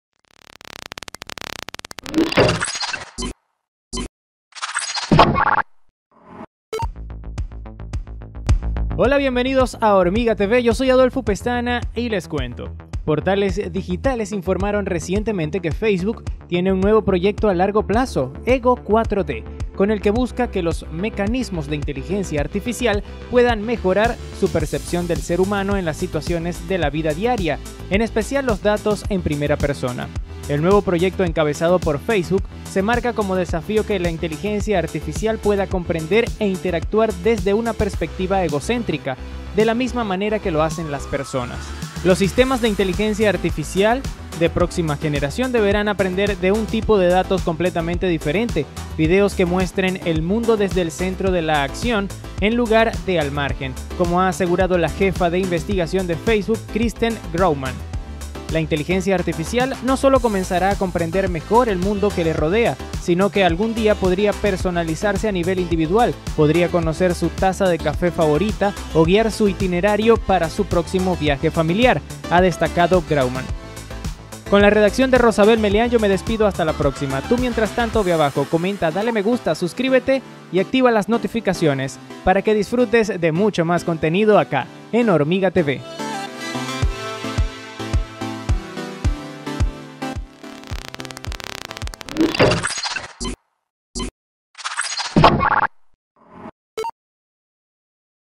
Nuevo proyecto de Facebook Ego4D, descubre de qué se trata | HormigaTV
Facebook tiene un nuevo proyecto a largo plazo Ego4D, con el que busca que los mecanismos de Inteligencia Artificial (IA) puedan mejorar su percepción del ser humano en las situaciones de la vida diaria, en especial los datos en primera persona.

 El nuevo proyecto encabezado por Facebook se marca como desafío que la Inteligencia Artificial pueda comprender e interactuar desde una perspectiva egocéntrica, de la misma manera que lo hacen las personas.